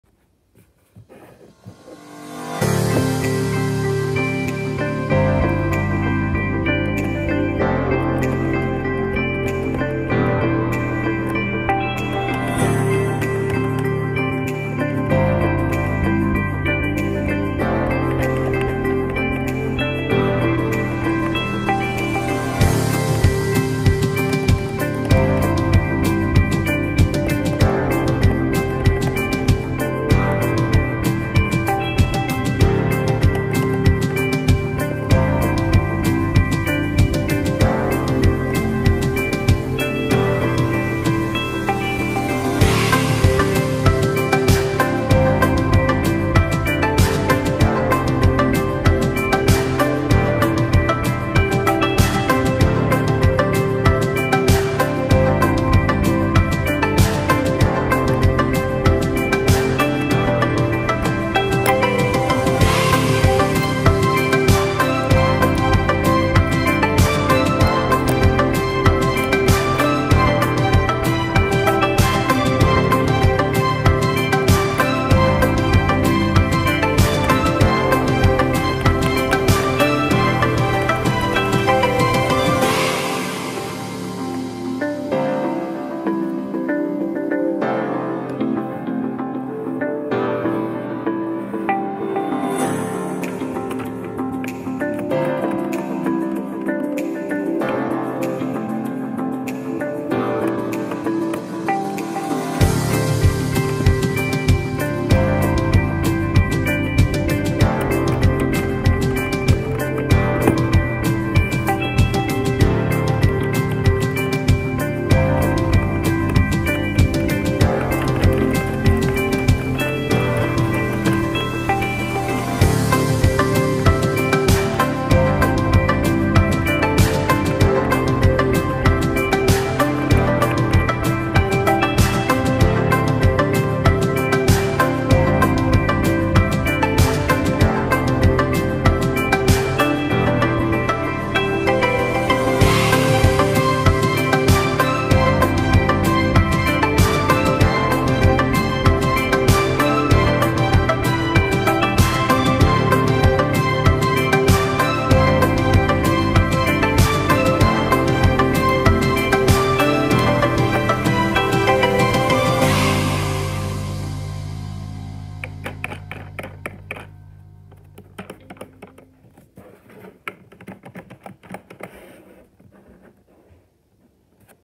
3 digit combo safe version 3
A 3 digit combo safe. No tutorial but mechanism video is included.